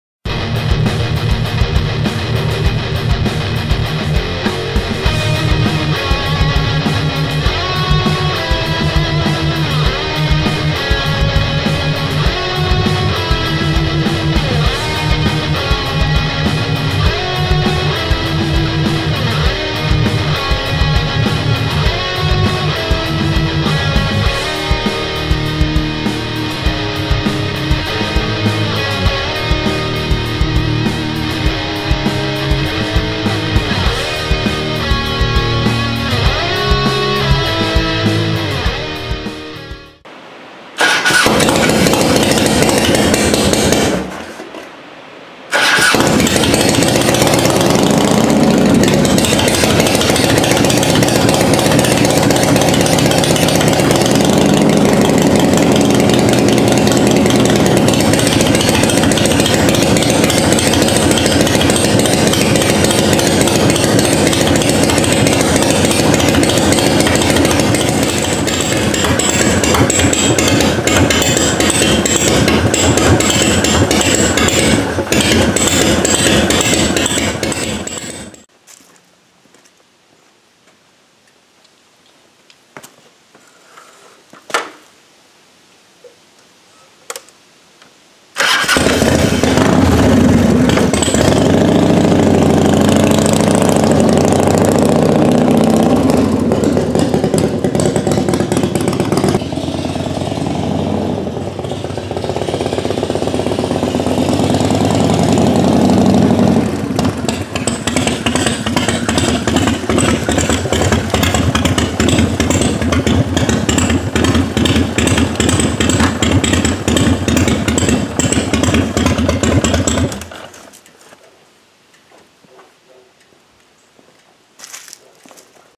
1989 Harley cold start
BK42 channel and a few other subscribers have been asking to see more of the Harley.
I had fun playing around with how to produce this video, I hope you enjoy it too!